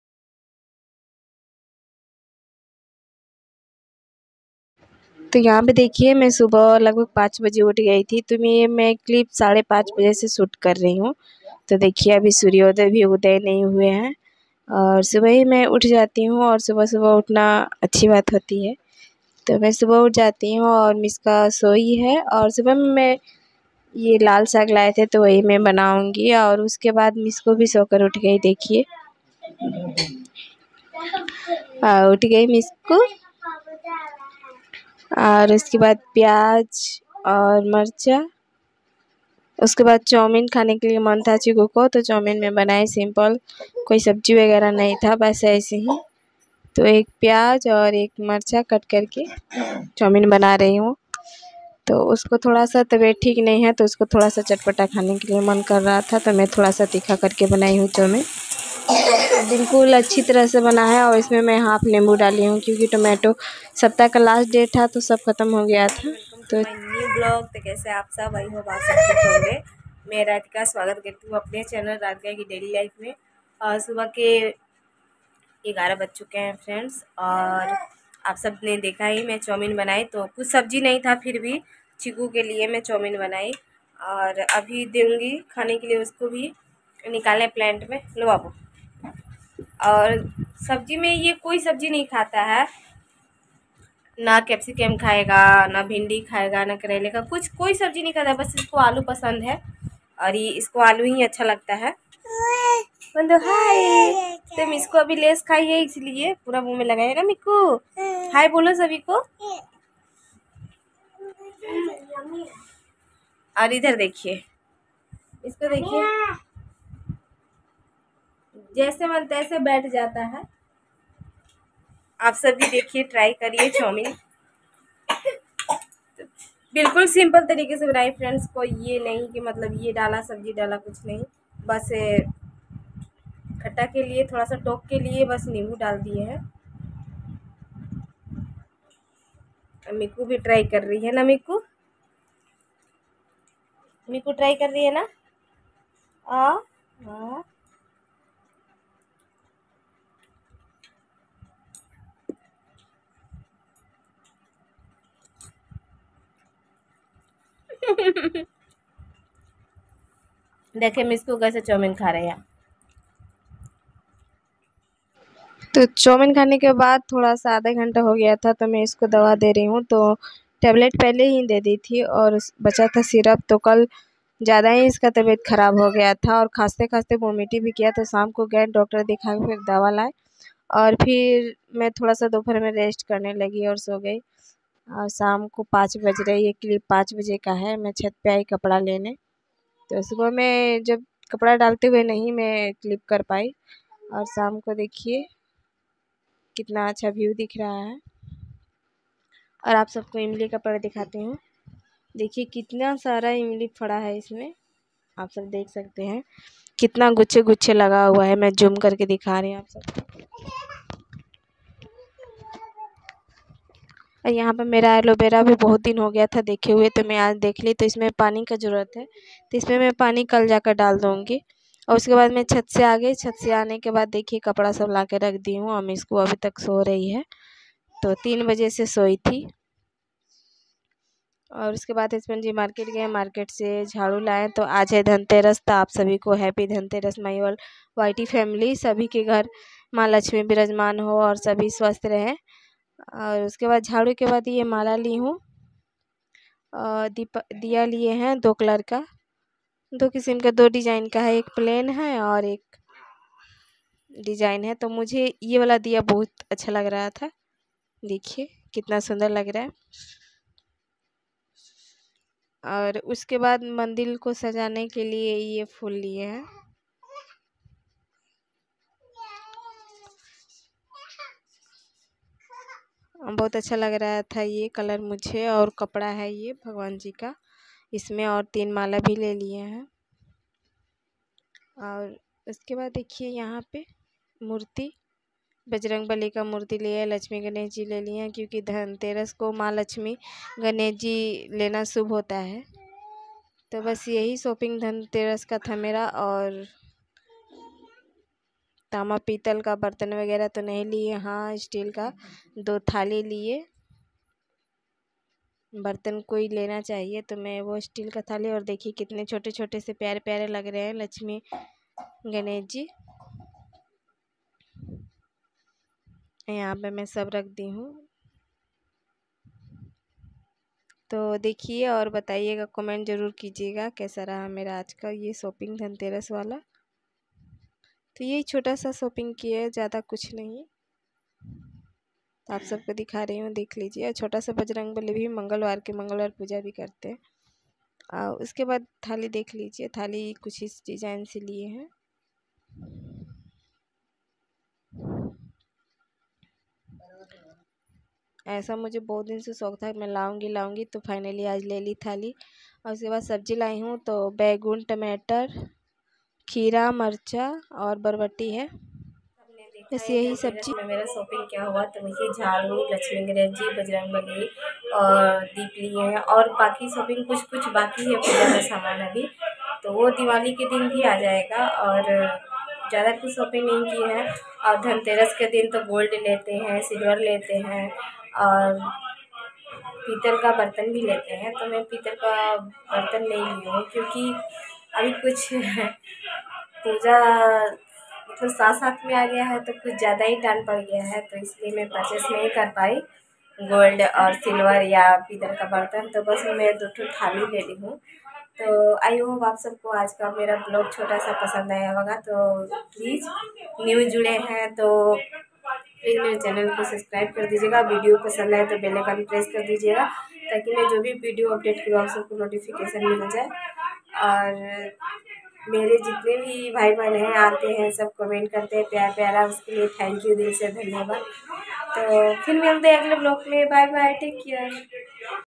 सुबह ☀️5 बजें से उठ कर सारा काम निपटा देती हुं || देखिए धनतेरस के दिन क्या खरीदी में ||🙏dailyvlog
सुबह ☀️5 बजें से उठ कर सारा काम निपटा देती हुं || देखिए धनतेरस के दिन क्या खरीदी में ||🙏